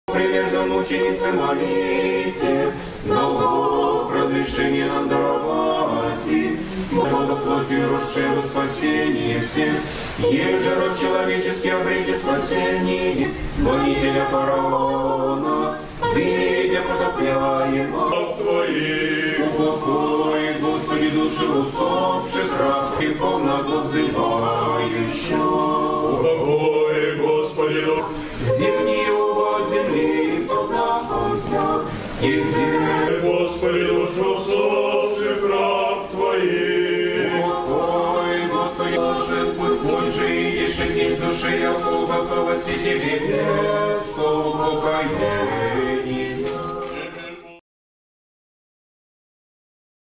Кемерово: похороны. Агарковы
Сергей и Наталия Агарковы потеряли в пожаре своих детей и их бабушку - в Кемерове сегодня проходят похороны 13 человек.